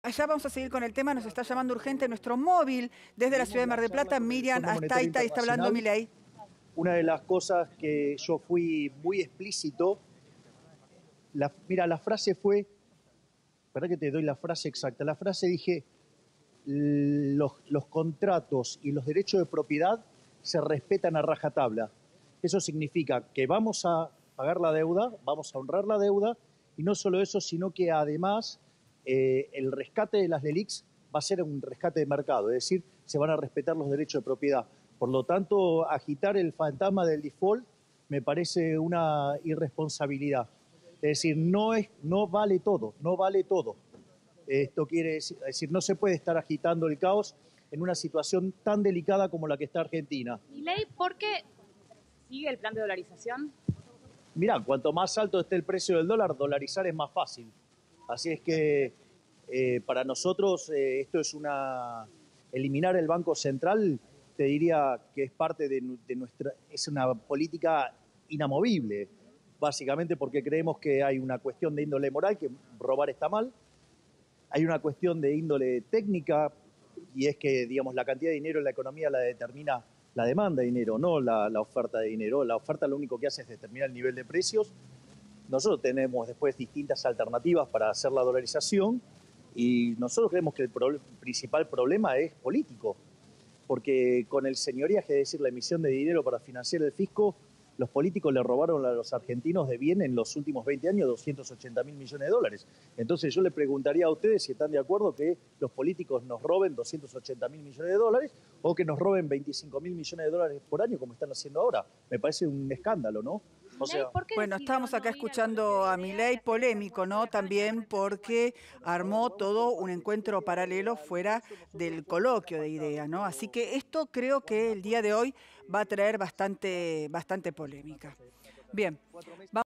Encuentro "paralelo" de Milei con empresarios: "Más alto el precio del dólar, más fácil dolarizar"
El candidato presidencial de La Libertad Avanza no asistirá al Coloquio de IDEA y se verá de forma paralela con otros representantes del círculo rojo. Dónde será el mitin y quiénes estarán a su lado.